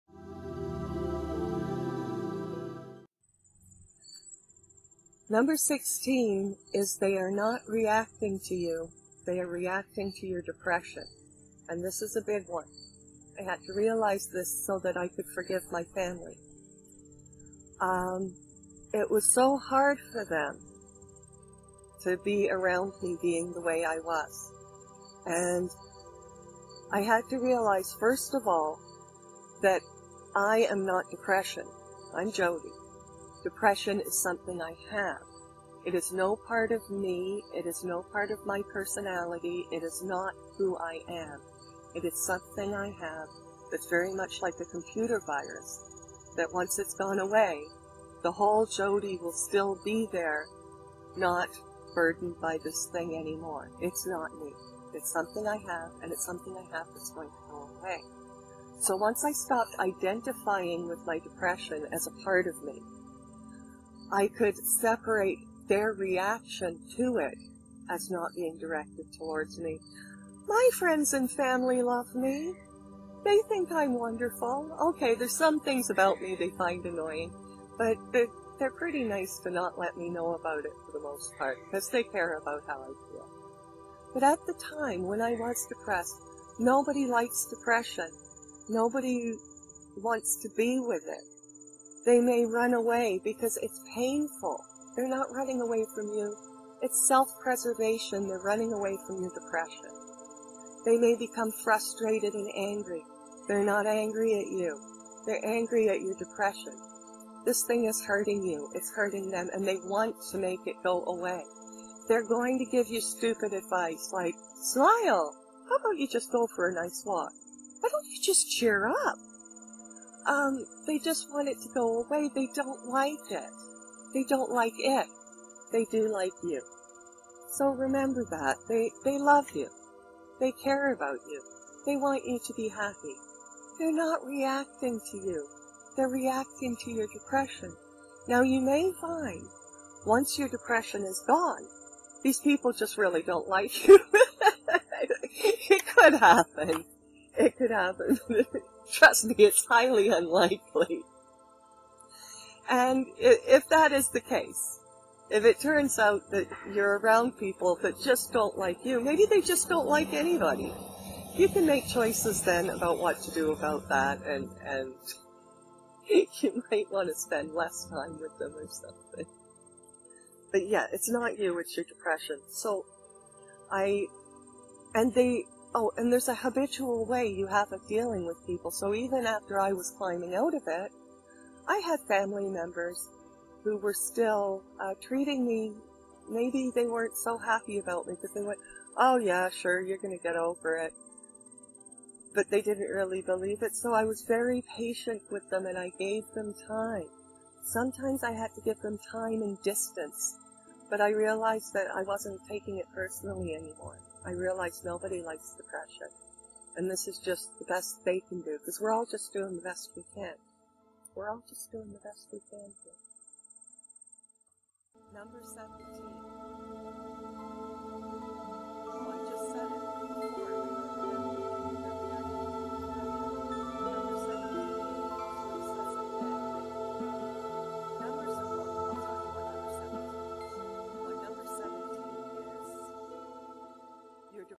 Depression Recovery #16 They Are Not Reacting To You Self Healing Help
They are reacting to your depression, and this is not who you are. I explain here how realising that those around me were reacting to my depression, and not to me, helped me heal my relationships.
I would also like to leave a little note here explaining why the distressed audio. I had discarded several earlier attempts because, for some odd reason, my neighbors feel the need to mow their lawns, or use other loud machinery, while I'm recording. I like this front garden area, so I decided to just go ahead with it, remove as much of the noise as I could in editing, and try to disguise the rest in some music. It's cold out now, so I'll be filming inside for the next few months.

Become my friend on Facebook.

An honest review of a Tony Robbins book and other fun stuff to read at my blog.